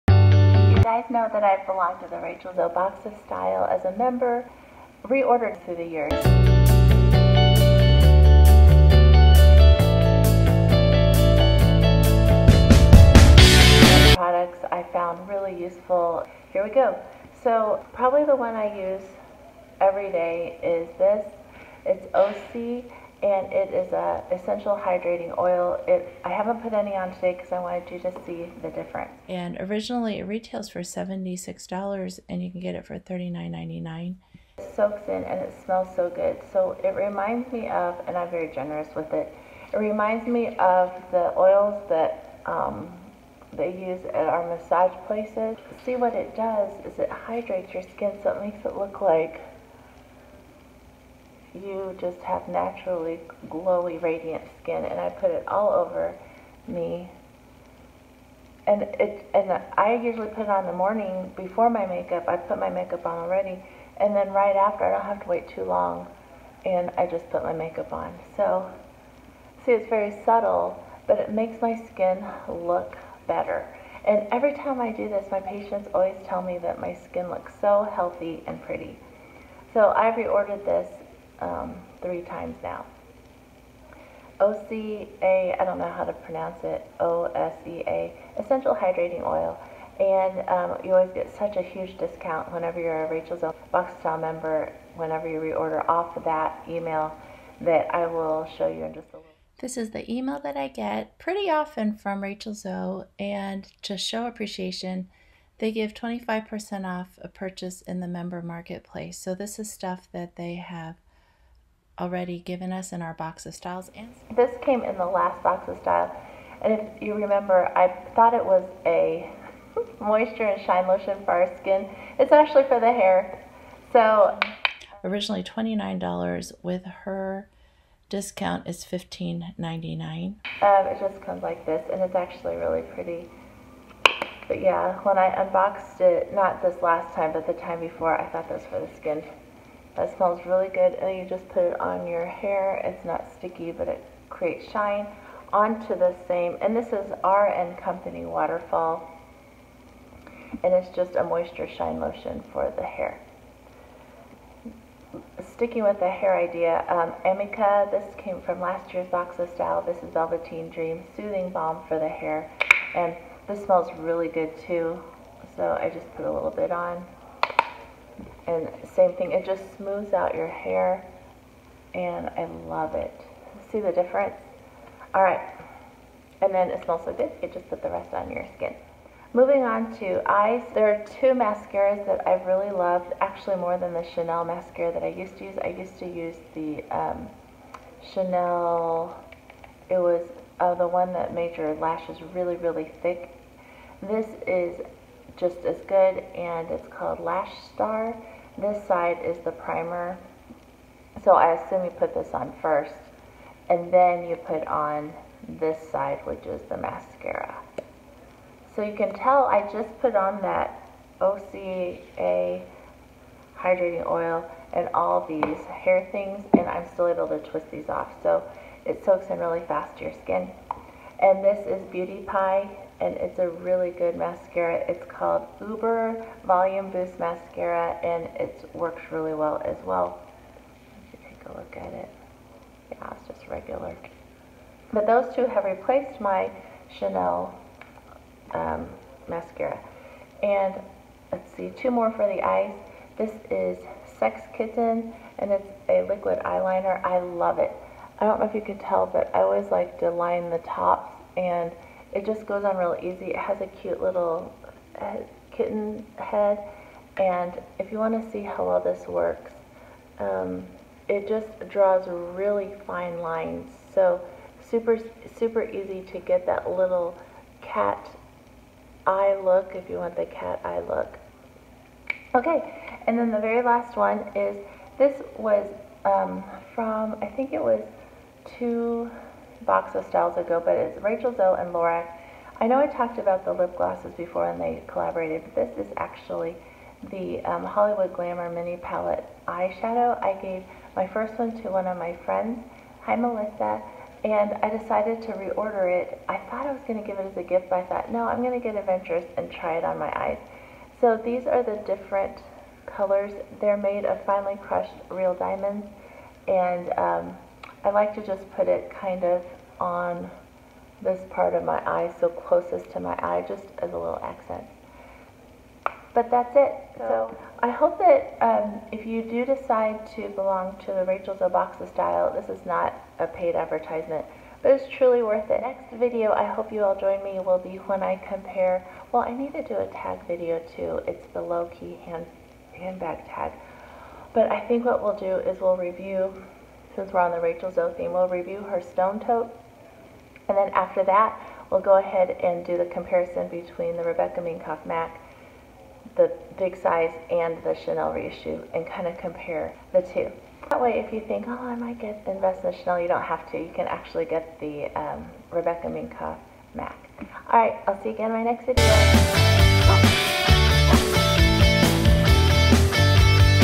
6 BEST BEAUTY PRODUCTS FROM RACHEL ZOE BOX OF STYLE | SEE HOW MUCH MONEY I SAVE!
As a Rachel Zoe Box of Style Member for the past 3 years, I have continued to reorder these 6 beauty products and have been so thankful for the savings and special extra discount codes Rachel Zoe gives us as well.  XO, Jen